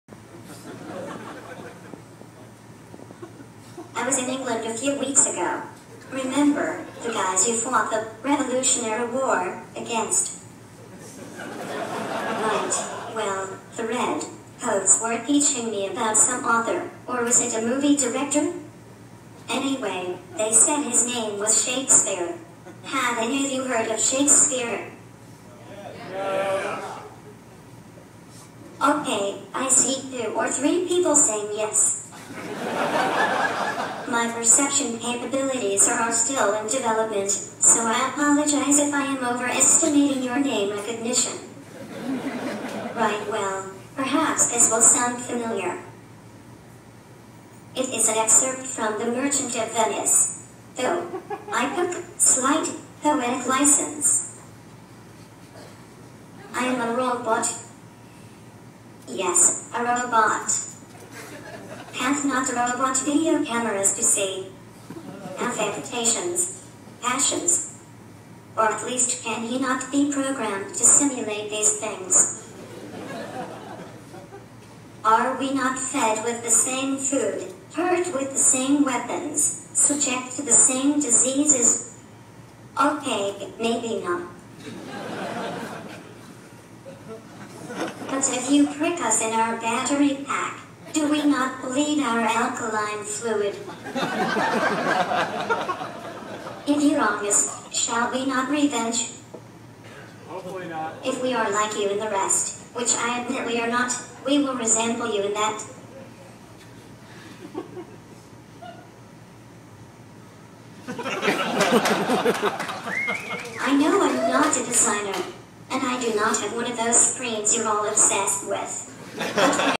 Heather Knight's Data
A joke-telling robot. Well sure. It's the eyeo festival, why not?